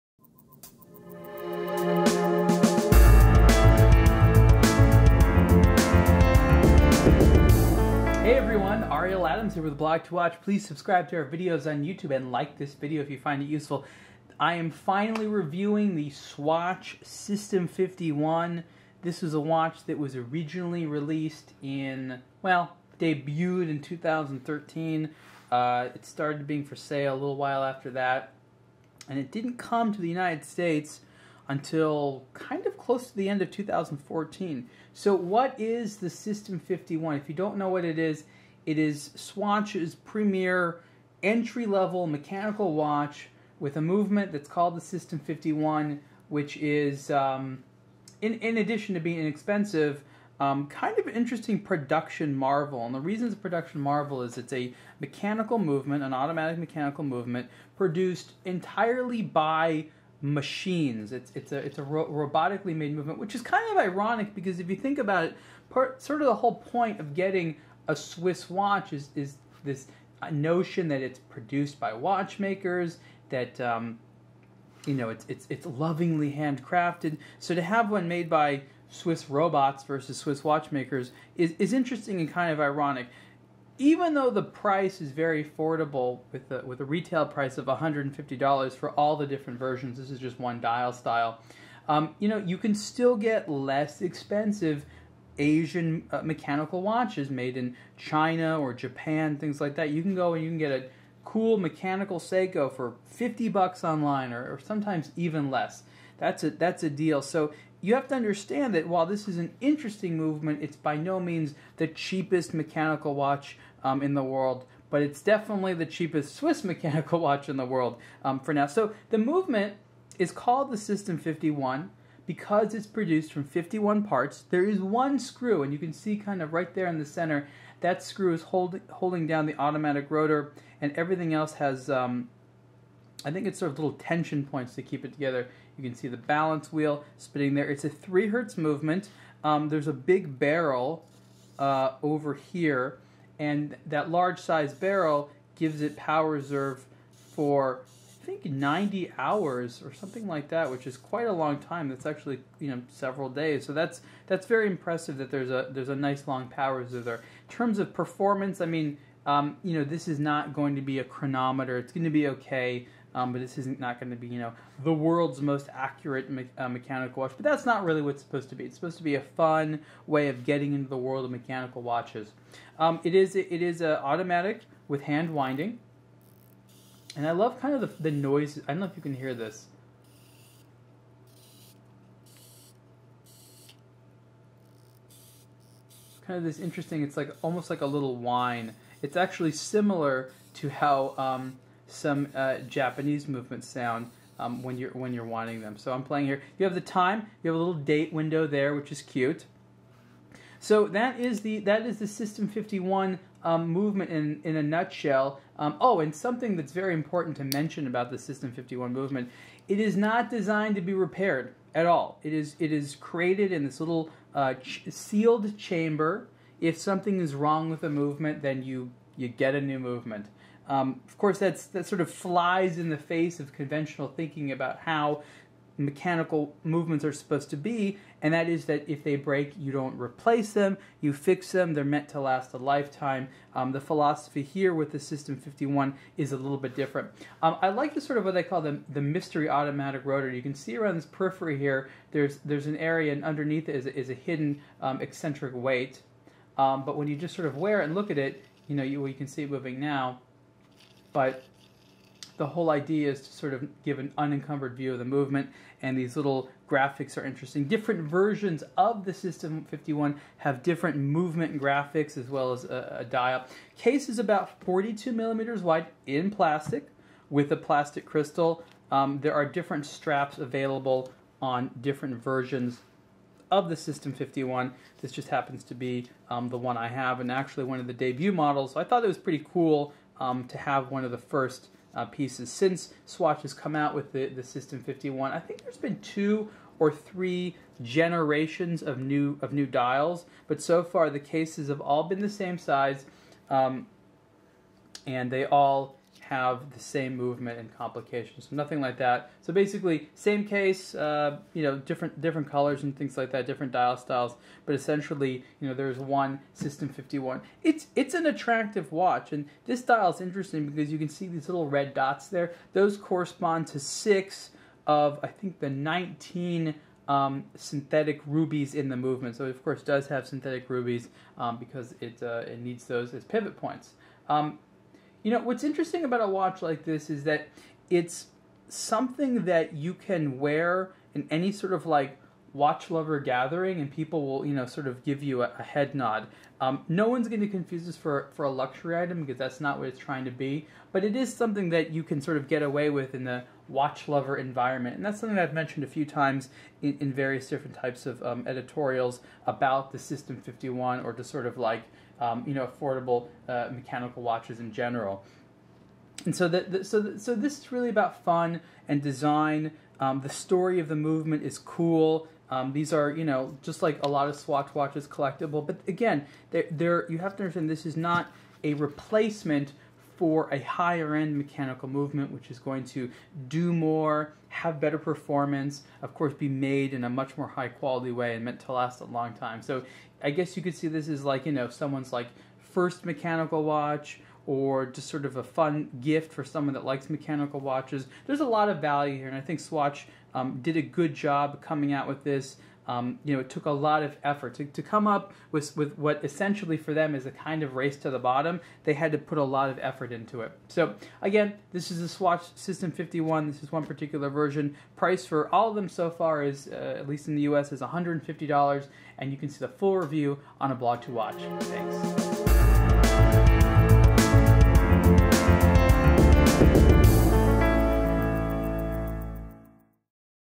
Swatch Sistem 51 Watch Review | aBlogtoWatch
Swatch Sistem 51 Watch Review

Ariel Adams of aBlogtoWatch reviews the $150 Swatch Sistem 51 watch.